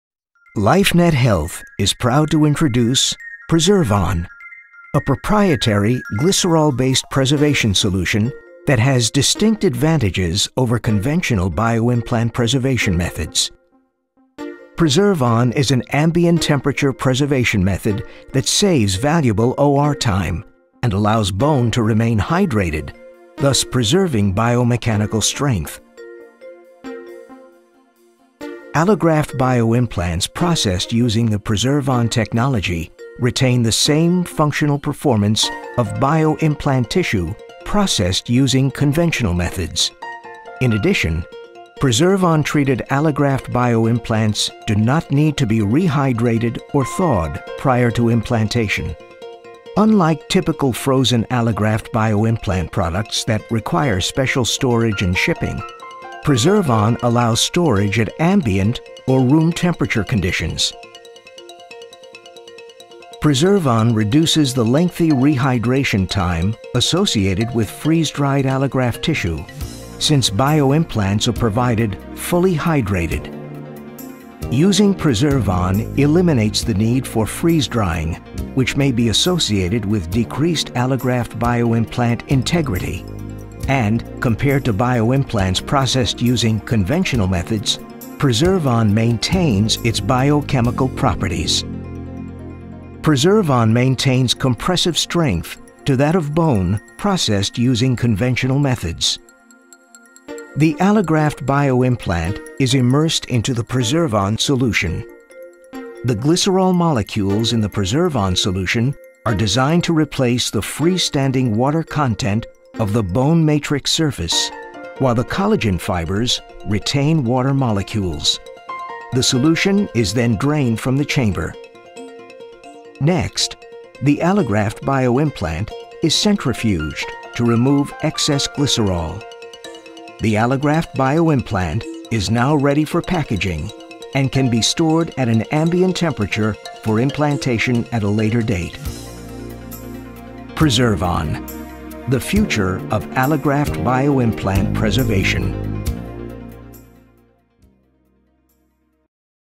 LifeNet: Preservon
This 3D animation is an example of Nucleus's custom animation projects for biotechnology, showing the ambient temperature preservation of tissue for Preservon.